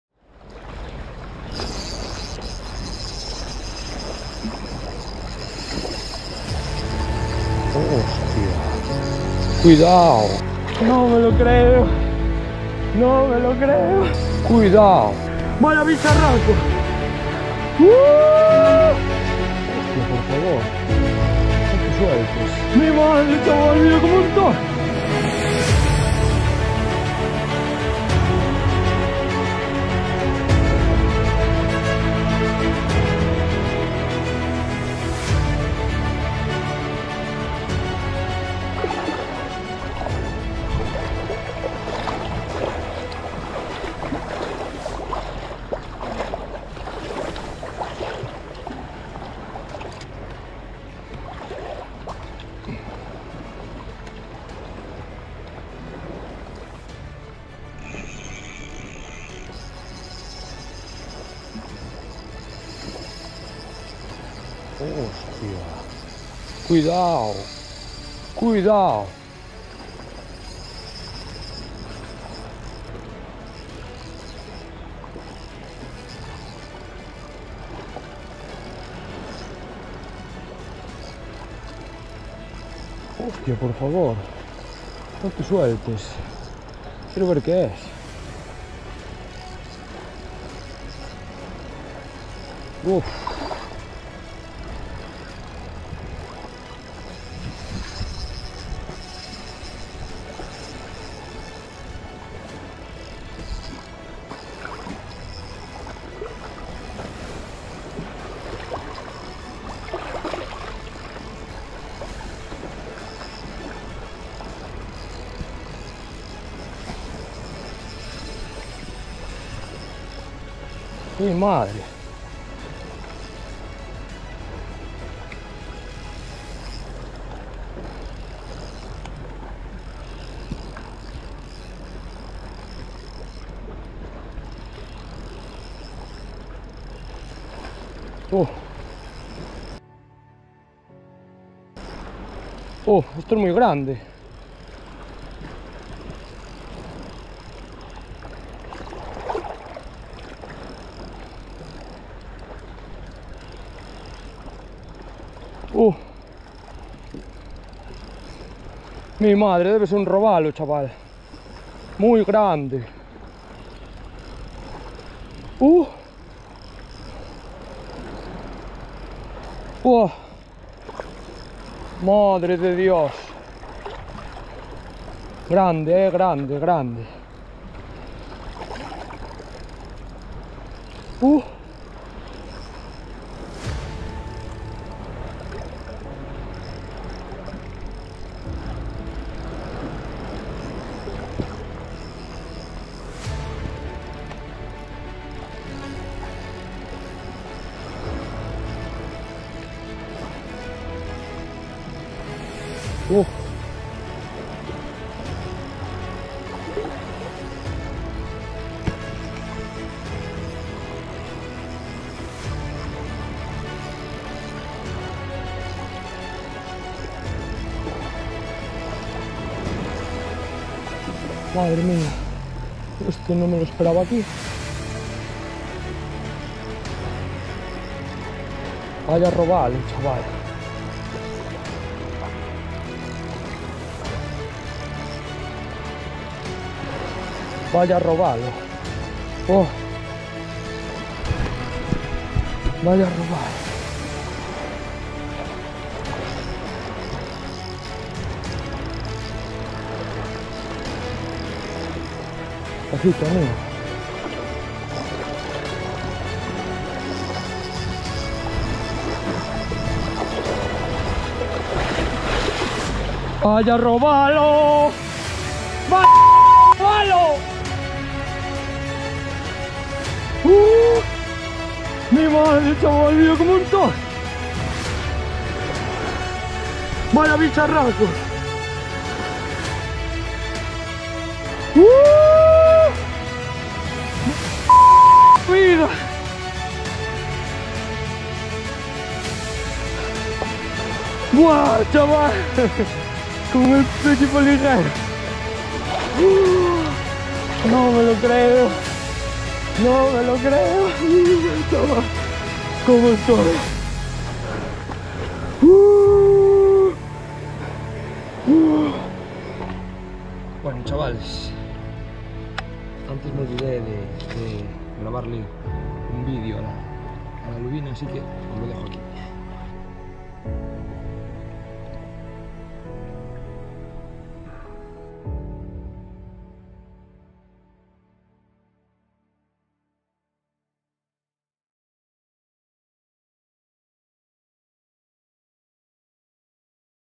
Pesca de lubina en la ria 2022 //ATAQUE MUY GORDO//
EQUIPO UTILIZADO:

-Caña major craft CEANA 762UL 2-10g
-Carrete shimano stradic fl hg 2500
-Trenzado: J-Braid Grand X8 0,06mm
-Bajo de línea: Fluorocarbono sunline black stream 0.26mm

Muy buenas amigos. Dejo  por aquí el video en el que capture un precioso robalo de 63cm y 2.8kg.
Un día claro en el que salieron unas cuantas lubinas al amanecer pero de pequeño porte. Me encontraba de regreso caminando lo que ya había pescado para poder salir al coche e irme a mi casa. En los sitios que mas me gustaban o mas llamaban mi atención echaba unos lances. Justo cuando ya casi estaba llegando a mi punto de inicio de jornada llego la sorpresa💣💣💥💥💥💥.

Espero que os guste amigos🤟🏻🤟🏻🤟🏻🔥🔥🔥🎣🎣🎣